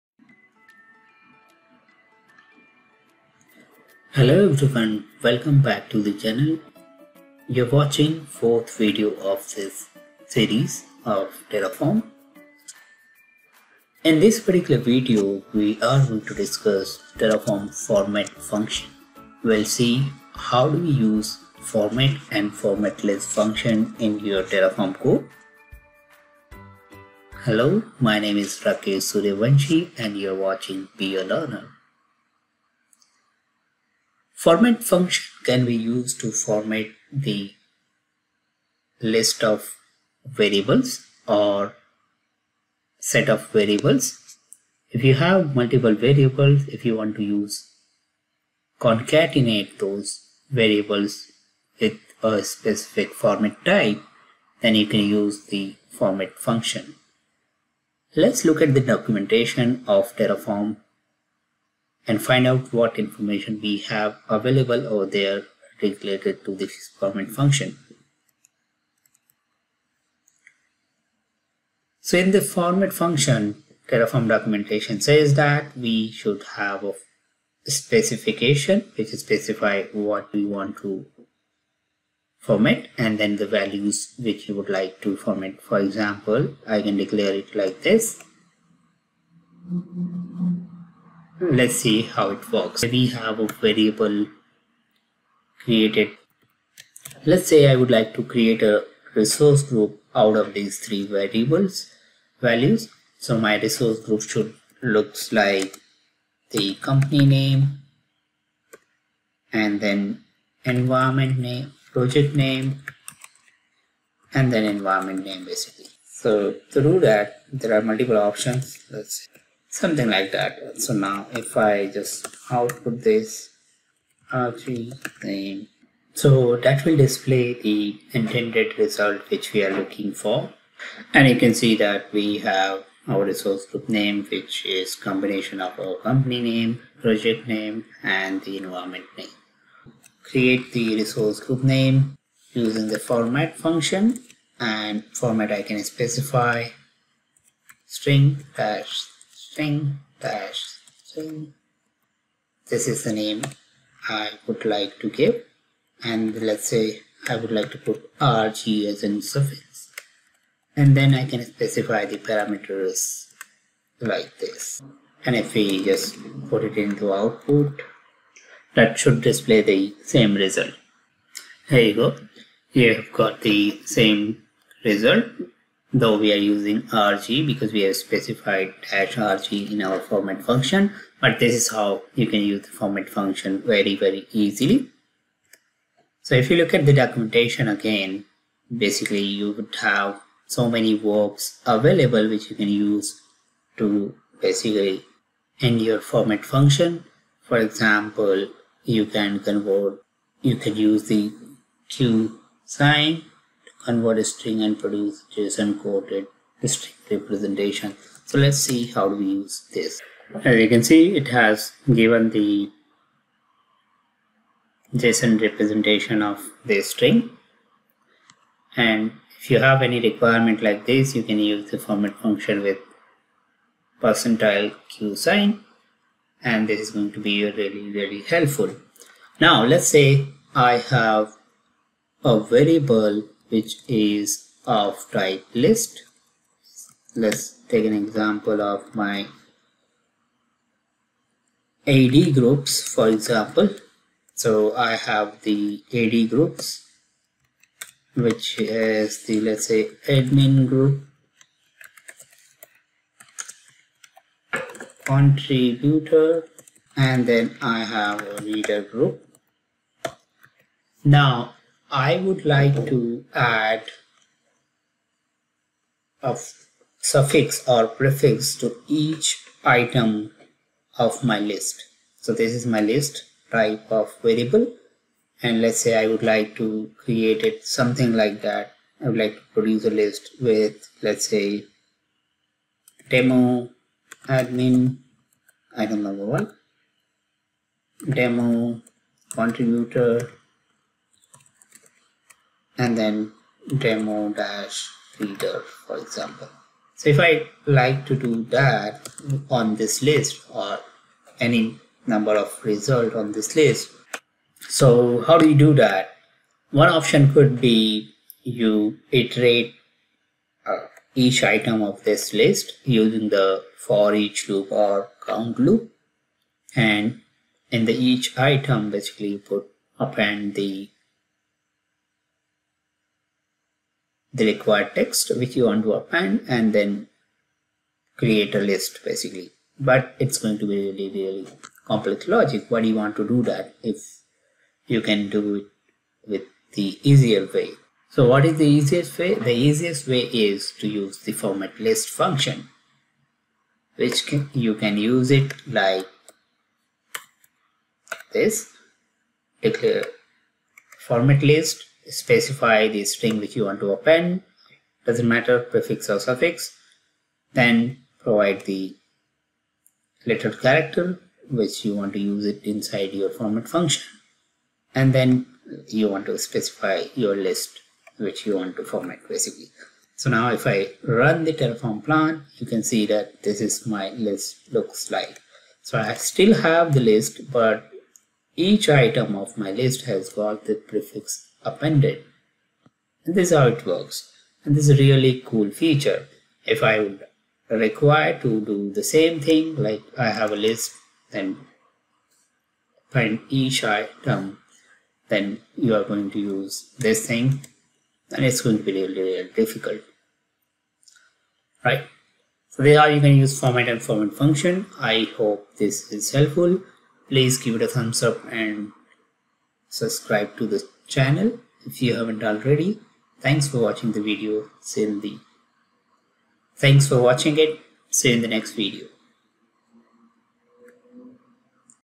Learn terraform | terraform format function
In this video you will learn how to use terraform format and formatlist function.

Table of Contents:

00:00 - Introduction
00:32 - format function definition
03:52 - using formatlist function